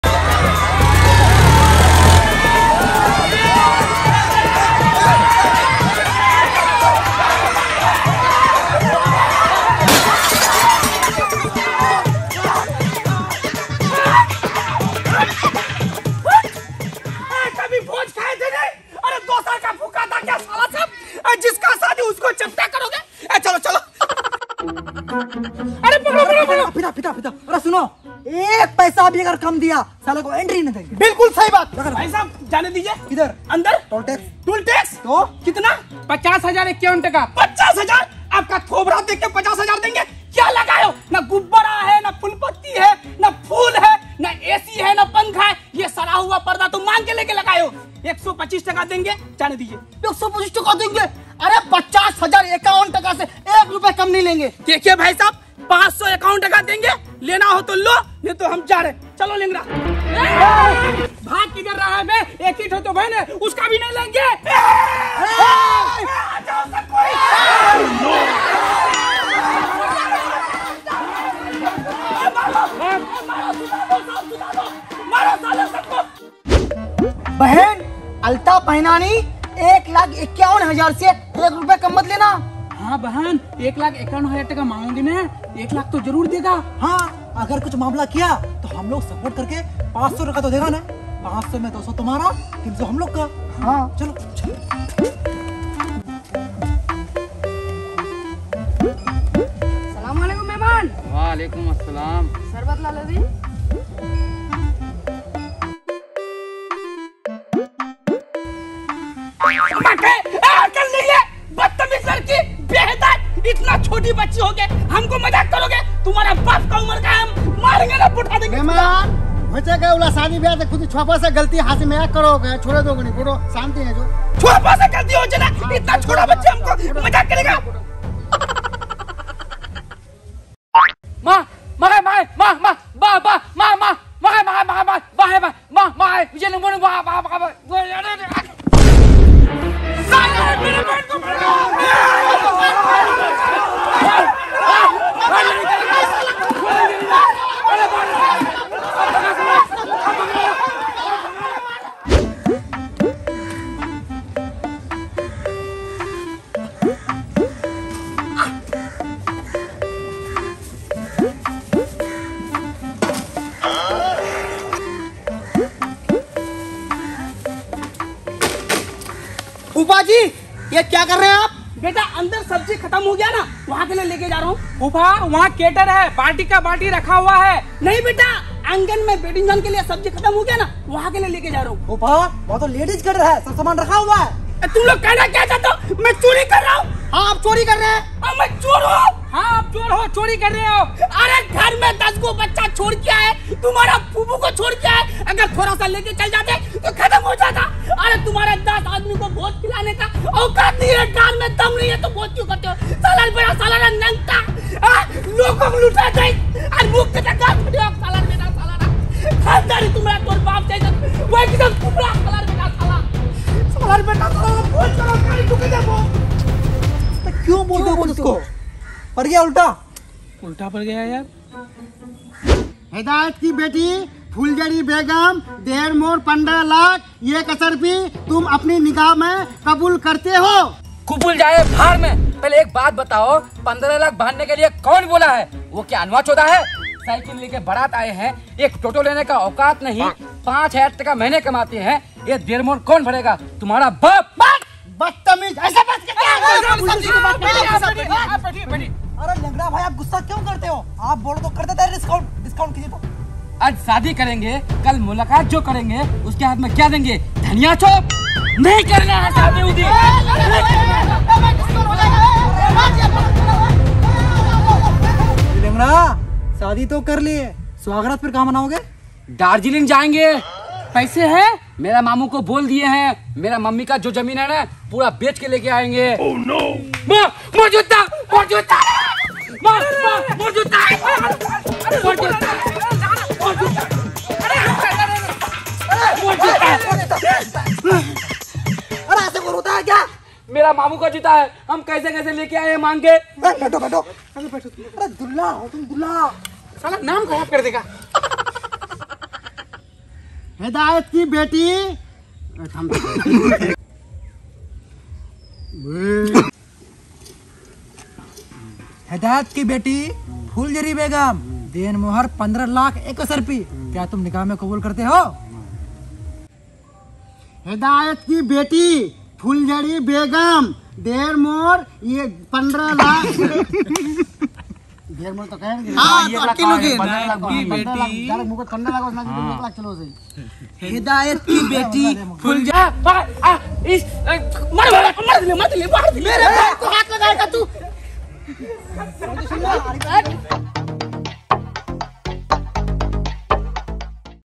Indian Shadi || The Jhamela Return || New Funny comedy video || Bindas Fun Heroes
Directed By - Javed and Team
Story            - Team
Editing By   - Javed Akhter
Camera        - Aaquib And Team

Actors    - Javed ,Shehnawaz, Manaur, Arif,Afroj, Mallu, Jhalsu

This is Surjapuri Hindi mixed Comedy Video.
This video is  made for only entertainment purpose .We don't wanna hurt anyone's feeling . We make funny video for entertainment only.